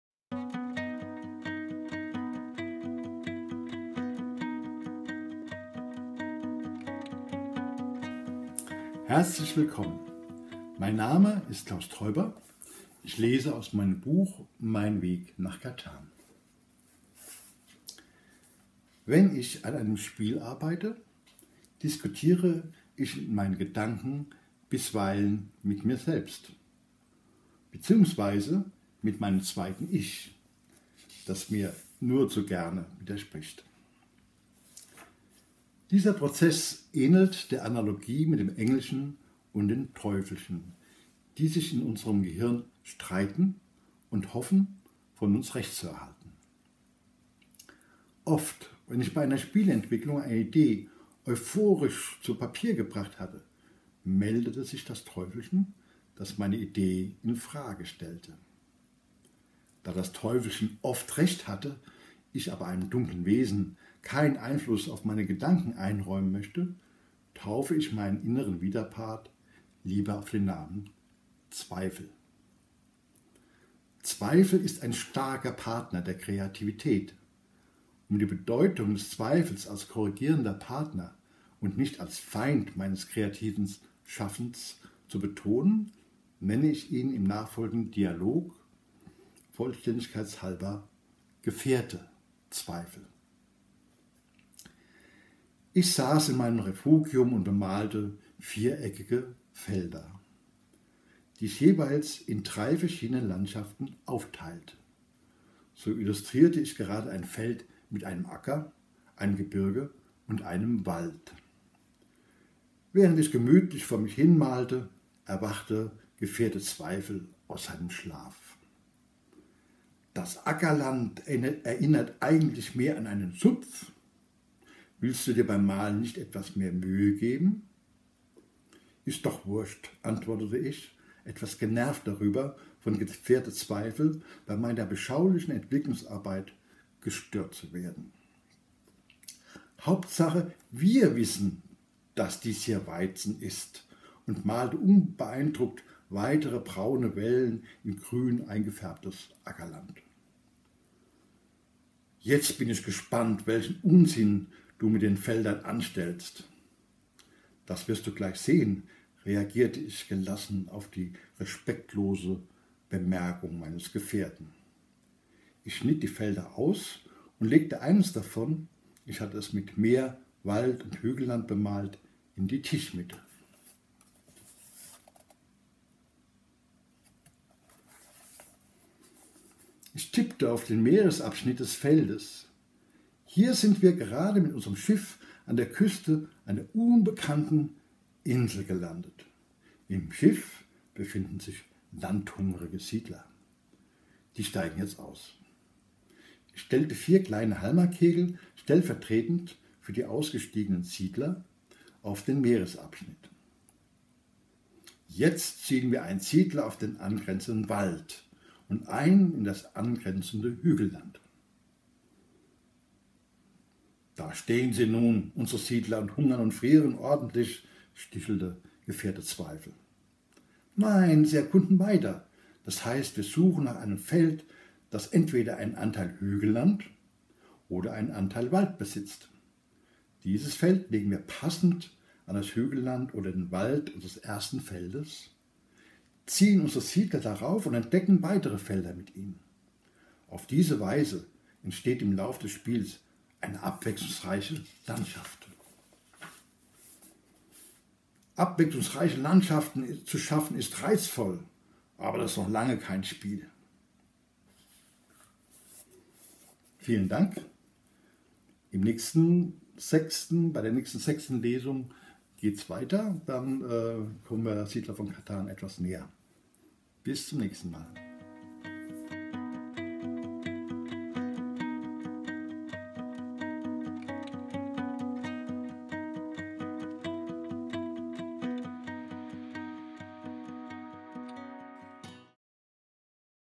My Journey to Catan – "My Doubt Companion" | Read by Klaus Teuber
Don't let your inner doubt get you down. In this excerpt, Klaus Teuber illustrates how he battles with doubt to make better games.
Here's the fifth special sneak peek of My Journey to Catan by Klaus Teuber, read by Klaus himself.

My Journey to Catan is being published for the first time in English in November 2020 by Aconyte Books. This exclusive edition is a slipcovered hardback book, bound in leatherette and individually numbered and hand signed by Klaus Teuber.